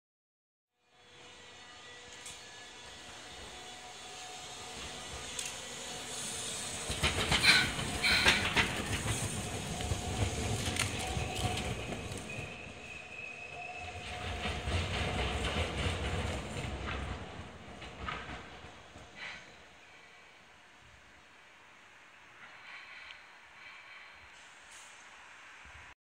広島電鉄800形810号 5号線広島駅行き 皆実町六丁目にて
広島電鉄の800形810号(千田車庫所属)です。撮影時は、平成28年の5号線の広島港発の一便目で走行していました。